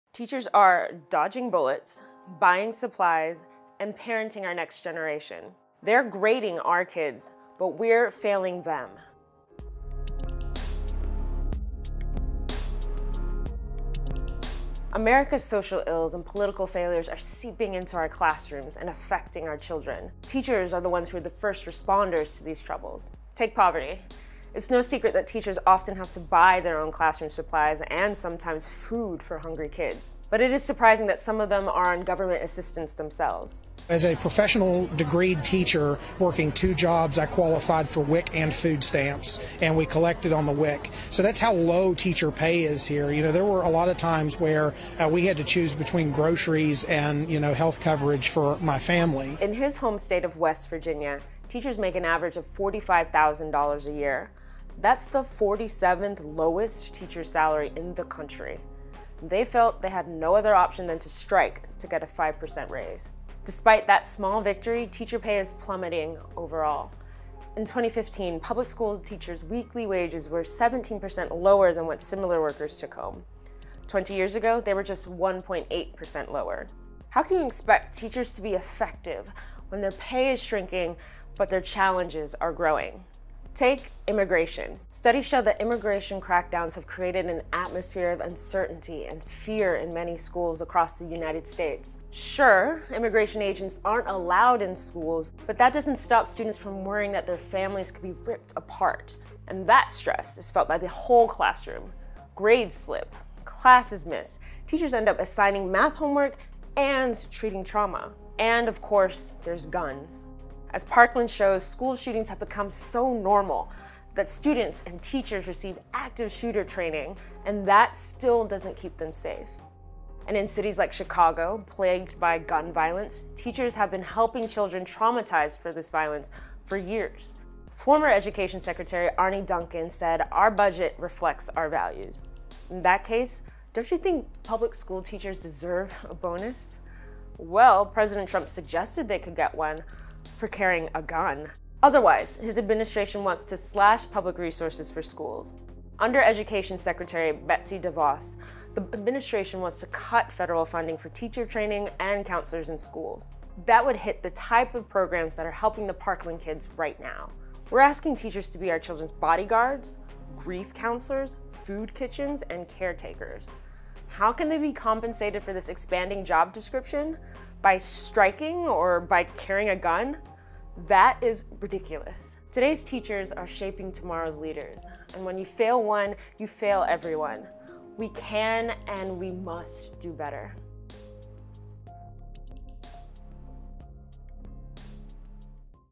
TL;DR: Teachers are America's hope, and we're failing them
Teachers are serving as bodyguards, therapists, soup kitchens and parents. And now we want them to carry guns, too? Wake up, America. We've got a problem. Opinion writer Karen Attiah sounds off.